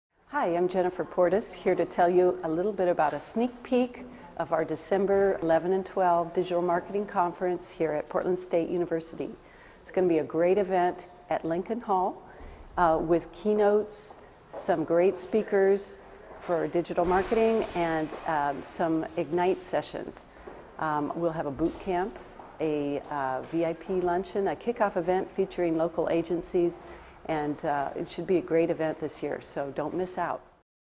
Jennifer Portis, Program Manager, Multimedia Professional Program, PSU
We caught up with Jennifer before the Digital Marketing Breakfast; Be sure to attend the Digital Marketing Conference December 11-12,